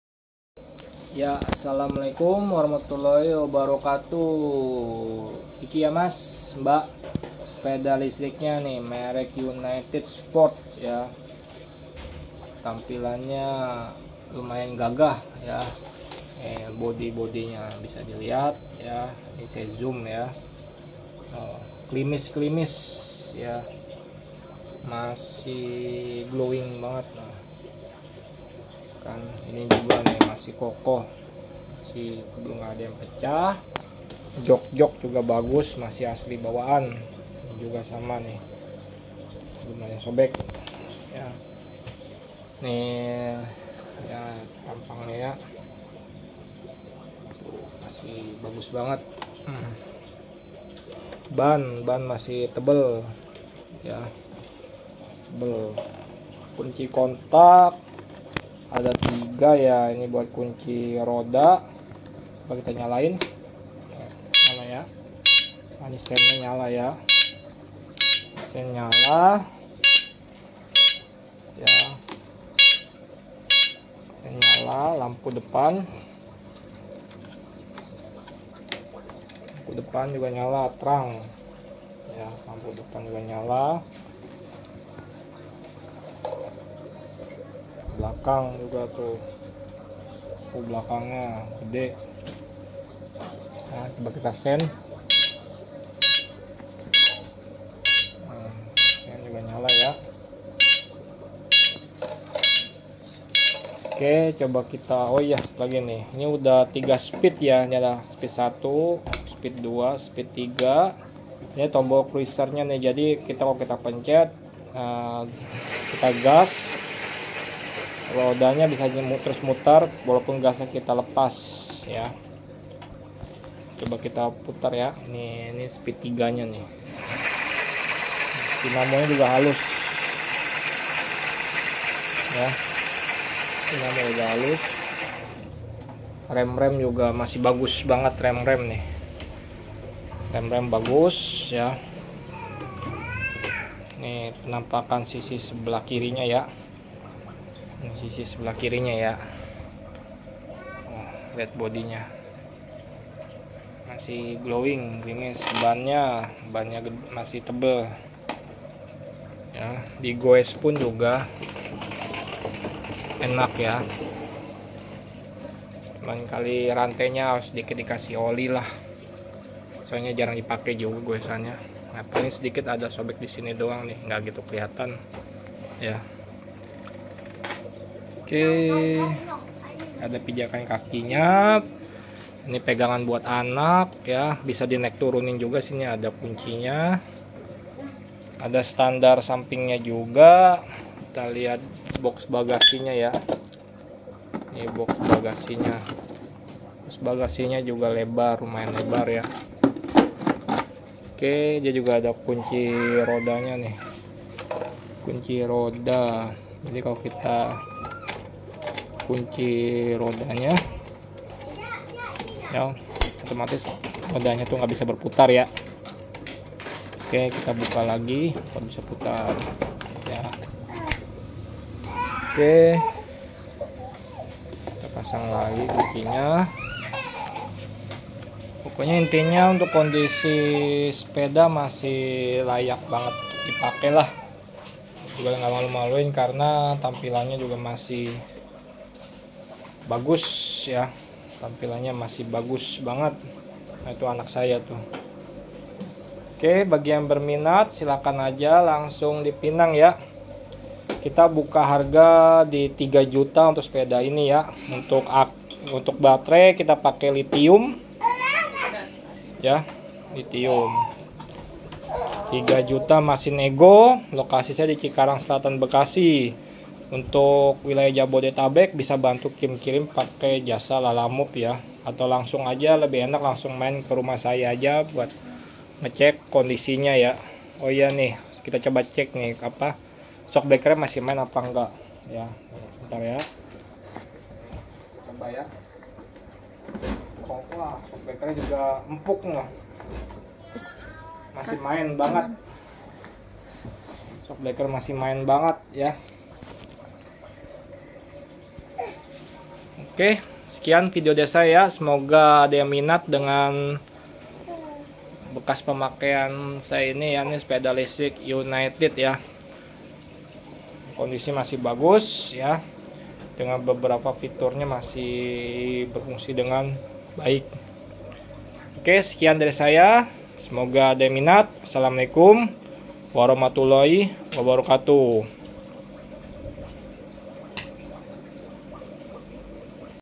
DIJUAL || Sepeda Listrik SELIS UNITY SPORT RED || SOLD OUT (Terjual)
Sepeda Listrik Merek UNITY SPORT

- Kondisi Mulus Klimis Siap Pakai Normal
- Baterai uda Lithium 58v 5.2ah
- Charger ada, Kunci Kontak & Kunci Roda
- Ada Alarm anti maling
- Kelistrikan normal, Lampu2 on smua
- Ban Super Tebel
- Unit siap pakai TANPA MINUS

Alhamdulillah Sudah TERJUAL (laku 2.7jt)